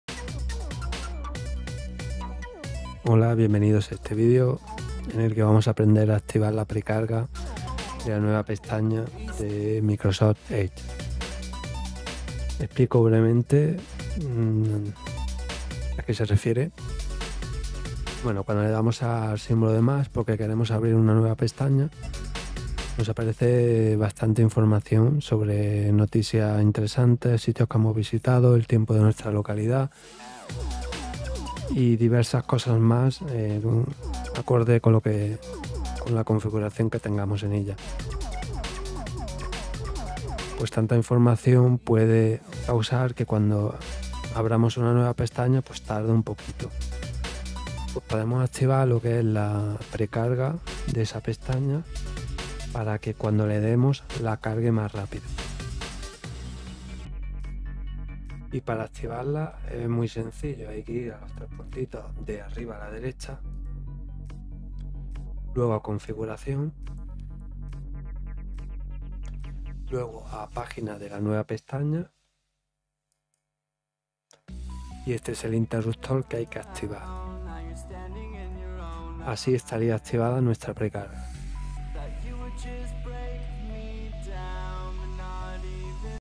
Cómo Activar la carga previa de pestañas nuevas en EDGE
Explico rápidamente en que consiste la carga rápida de pestañas nuevas en En el navegador Microsoft Edge y como activarla.